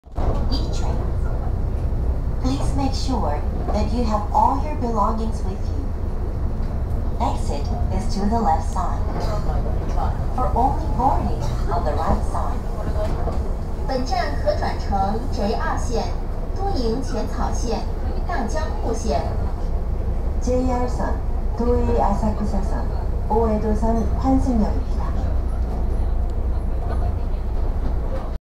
山手線E235系を撮った。田町駅ー浜松町駅
東京モノレール10000形
山手線田町駅ー浜松町駅にて